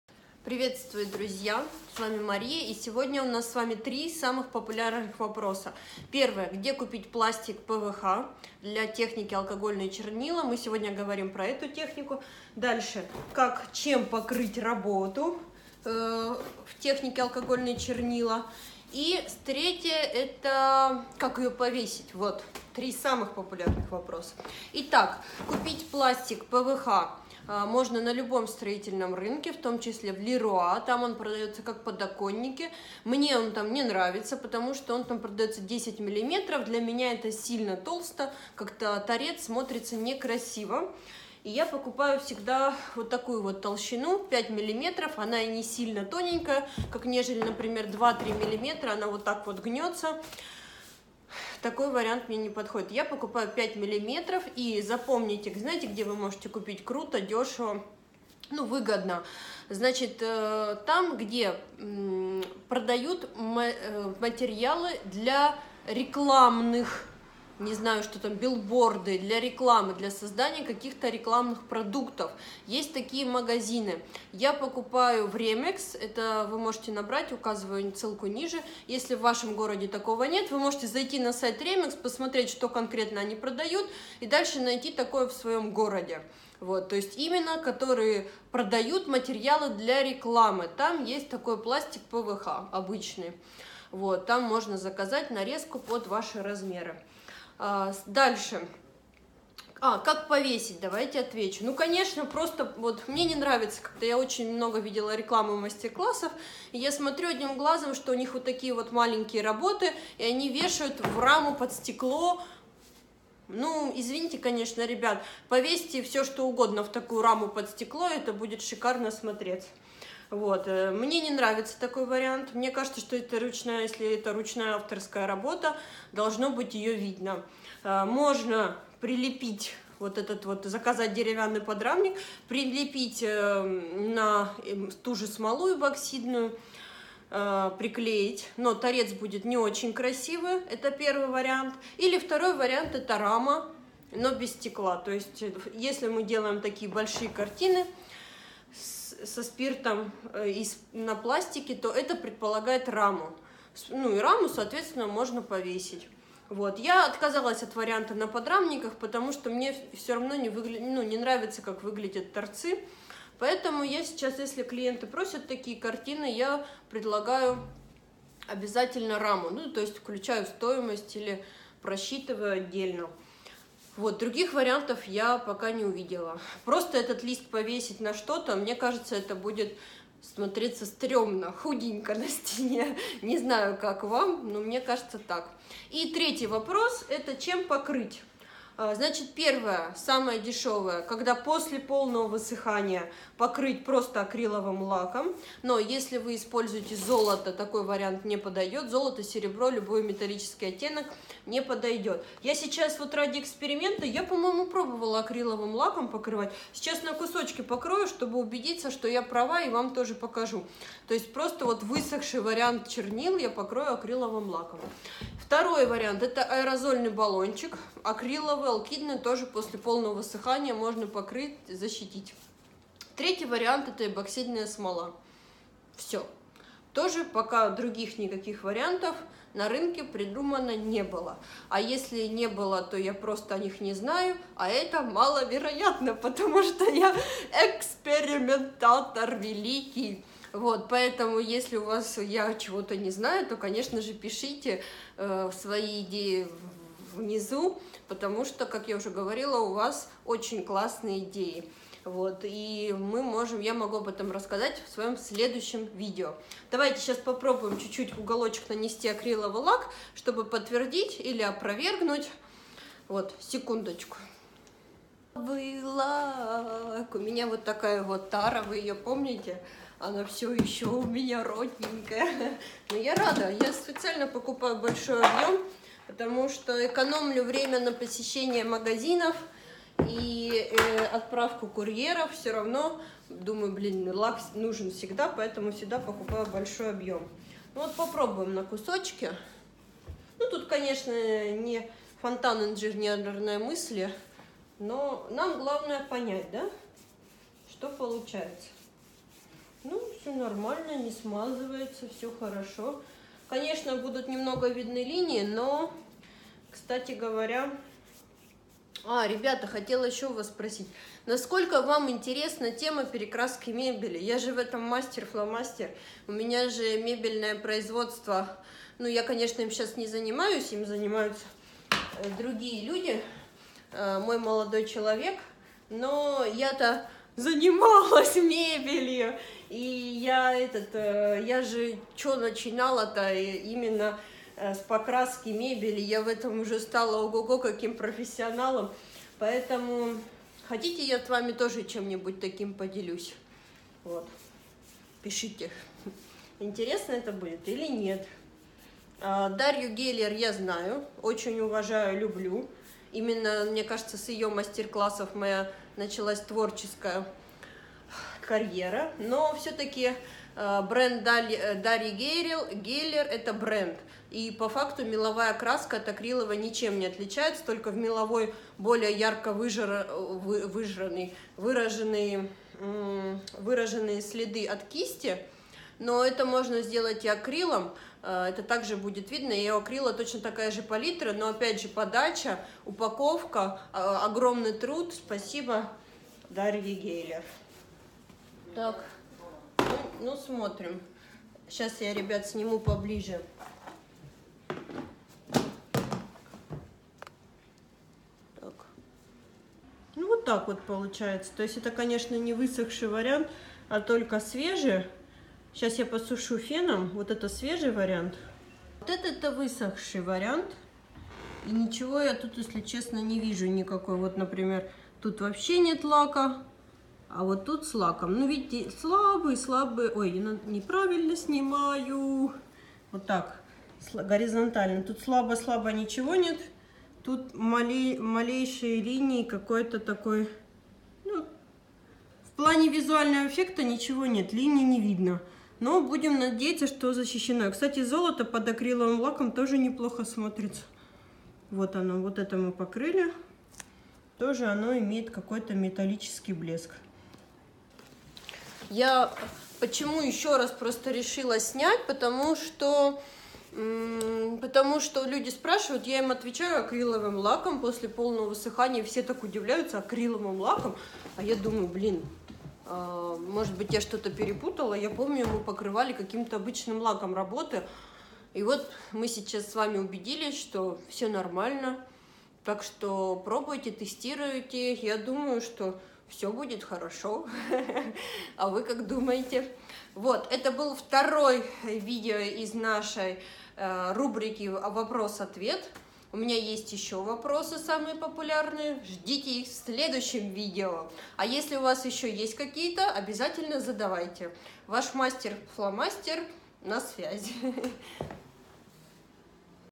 Алкогольные чернила! Где купить пластик выгодно? как повесить картину? какой лак нужен сверху?
Акогольные чернила! Где купить пластик выгодно? как повестить картину? какой лак нужен сверху?
итак это топ-3 самых популярных вопроса, которые приходят мне в директ!

ищите что-то подобное у себя в городе вперед!))

Так, про лак ответила, и даже показала, про повестить тоже!

И если еще есть вопросы - пишите - внесу их в списочек!)